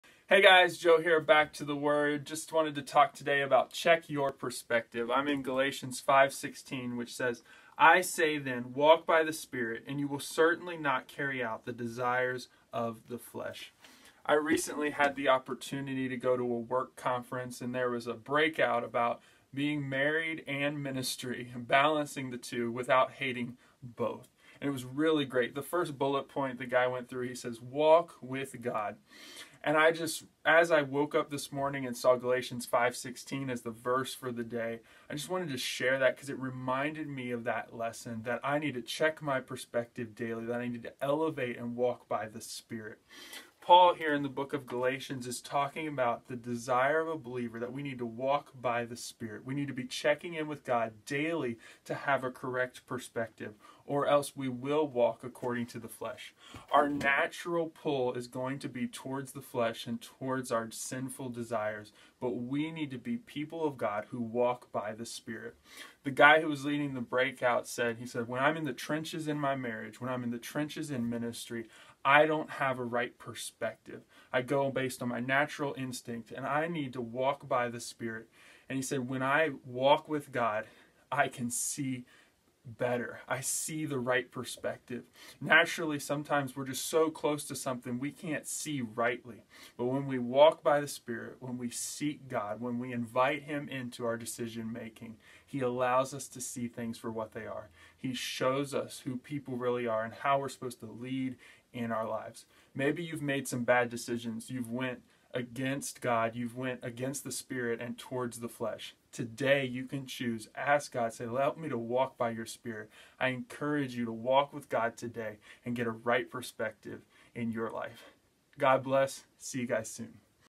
Right Choices // Galatians 5:16
I say then, walk by the Spirit and you will certainly not carry out the desire of the flesh. - ‭Galatians‬ ‭5‬:‭16‬ CSB

Will you choose the desire of the flesh? Or walk by the Spirit?